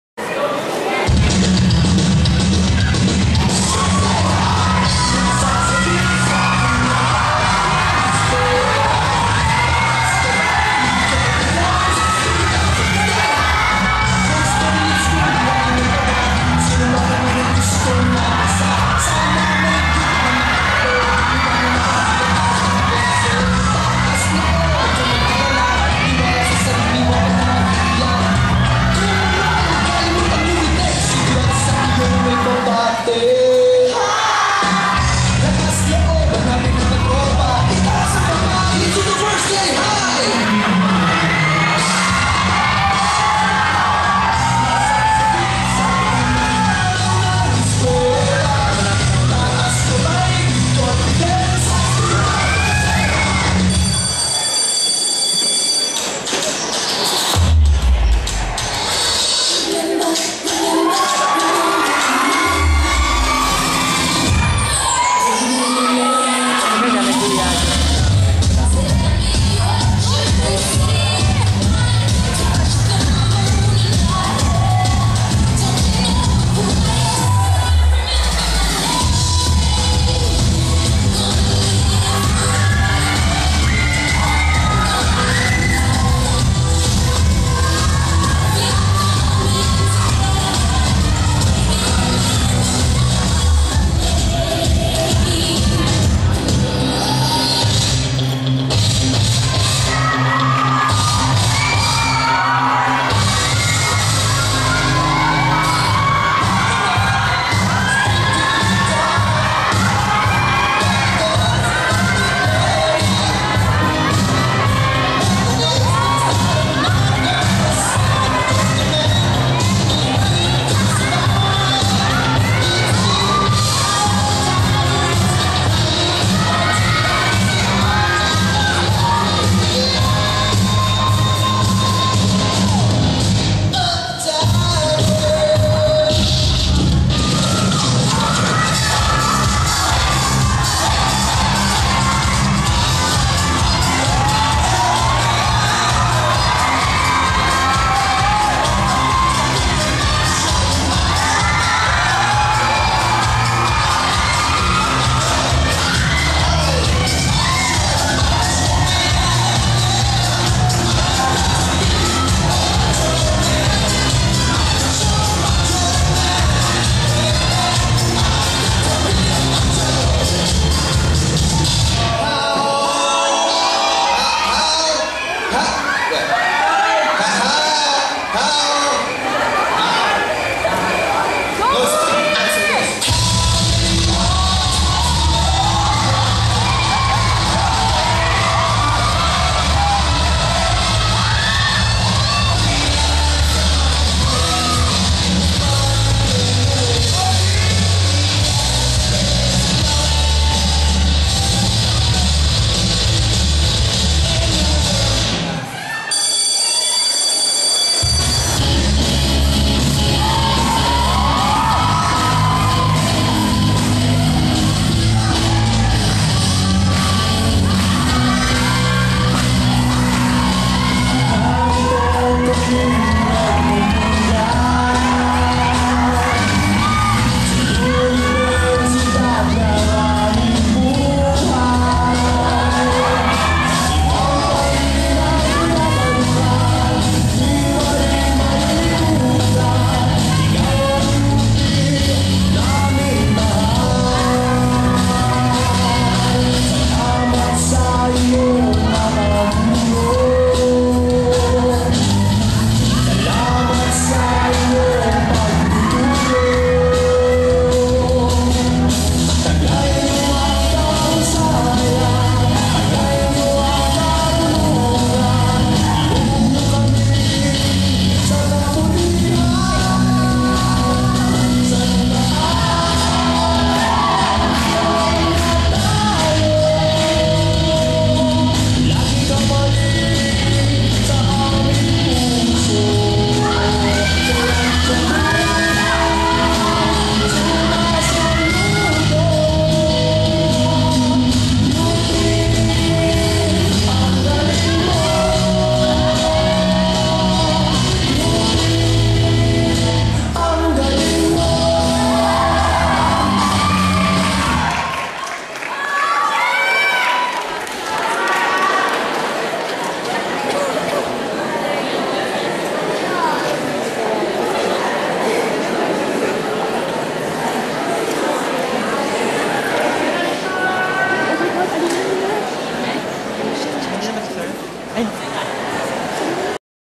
upm indayog Sayaw Manila 7 unibersiyudad UP Manila life.wmv
this is the 3rd dance in the unibersiyudad portion of Sayaw Manila 7 of the UP Manila Indayog Dance Varsity. Deseree, an innocent country lass finally meets the people in her new school UP Manila. She suffers culture shock, seeing the diversity found in the school.